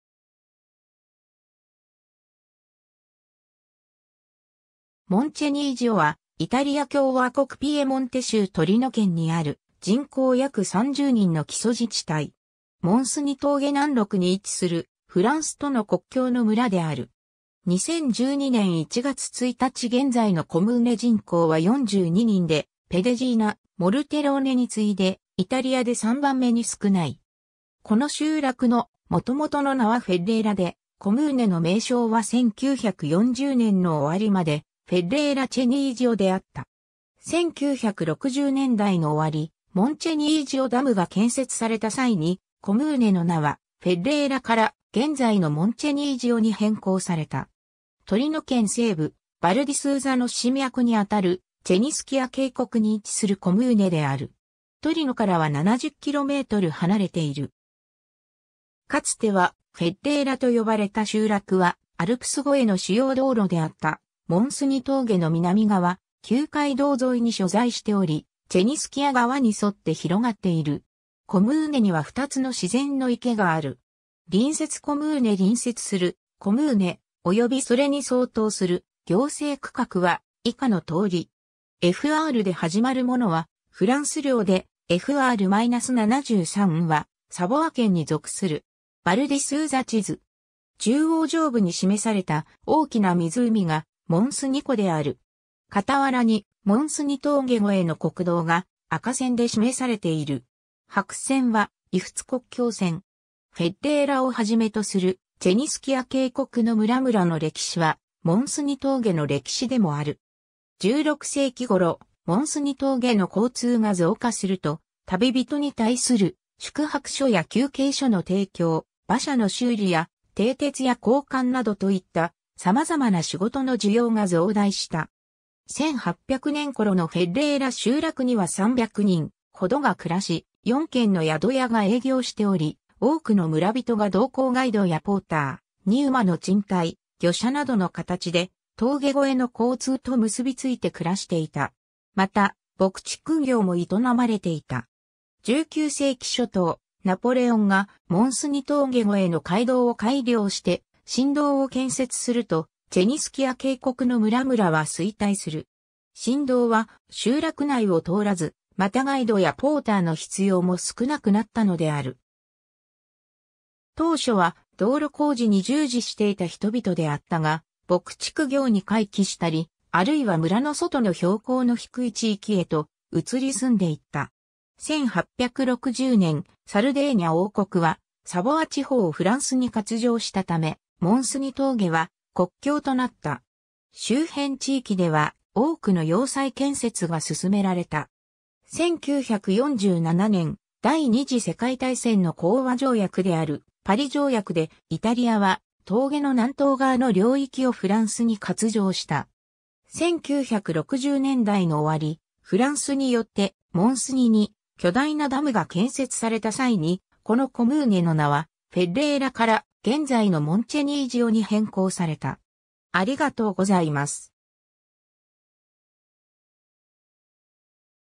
モンチェニージオ
モンチェニージオ（イタリア語: Moncenisio）は、イタリア共和国ピエモンテ州トリノ県にある、人口約30人の基礎自治体（コムーネ）。
 モン・スニ峠（モンチェニージオ峠）南麓に位置する、フランスとの国境の村である。
2012年1月1日現在のコムーネ人口は42人で、ペデジーナ（ソンドリオ県）、モルテローネ（レッコ県）に次いでイタリアで3番目に少ない。
 この集落のもともとの名はフェッレーラ（Ferrera）で、コムーネの名称は1940年の終わりまでフェッレーラ・チェニージオ（Ferrera Cenisio）であった。
 1960年代の終わり、モンチェニージオ・ダムが建設された際に、コムーネの名はフェッレーラから現在のモンチェニージオに変更された。
 トリノ県西部、ヴァル・ディ・スーザの支脈にあたるチェニスキア渓谷 (it:Val Cenischia) に位置するコムーネである。
トリノからは70km離れている。
 かつてはフェッレーラと呼ばれた集落は、アルプス越えの主要道路であったモン・スニ峠（モンチェニージオ峠）の南側、旧街道沿いに所在しており、チェニスキア川に沿って広がっている。
コムーネには2つの自然の池がある。
 隣接コムーネ 隣接するコムーネ、およびそれに相当する行政区画は以下の通り。
FRで始まるものはフランス領で、FR-73はサヴォワ県に属する。
 ヴァル・ディ・スーザ地図。
中央上部に示された大きな湖がモン・スニ（モンチェニージオ）湖である。
傍らにモン・スニ峠越えの国道が赤線で示されている。
白線は伊仏国境線。
 フェッレーラをはじめとするチェニスキア渓谷の村々の歴史は、モン・スニ峠（モンチェニージオ峠）の歴史でもある。
16世紀頃、モン・スニ峠の交通が増加すると、旅人に対する宿泊所や休憩所の提供、馬車の修理や蹄鉄や交換などといった、様々な仕事の需要が増大した。
1800年頃のフェッレーラ集落には300人ほどが暮らし、4軒の宿屋が営業しており、多くの村人が同行ガイドやポーター、荷馬の賃貸、御者などの形で峠越えの交通と結びついて暮らしていた。
また、牧畜業も営まれていた。
 19世紀初頭、ナポレオンがモン・スニ峠越えの街道を改良して新道（現在の国道25号線 (it:Strada statale 25 del Moncenisio) ）を建設すると、チェニスキア渓谷の村々は衰退する。
新道は集落内を通らず、またガイドやポーターの必要も少なくなったのである。
当初は道路工事に従事していた人々であったが、牧畜業に回帰したり、あるいは村の外の標高の低い地域へと移り住んでいった。
 1860年、サルデーニャ王国（翌年にイタリア王国となる）はサヴォワ地方をフランスに割譲したため、モン・スニ峠は国境となった。
周辺地域では多くの要塞建設が進められた。
1947年、第二次世界大戦の講和条約であるパリ条約で、イタリアは峠の南東側の領域（集落を除くこのコムーネの大部分）をフランスに割譲した。
 1960年代の終わり、フランスによってモン・スニ（モンチェニージオ）に巨大なダムが建設された際に、このコムーネの名はフェッレーラから現在のモンチェニージオに変更された。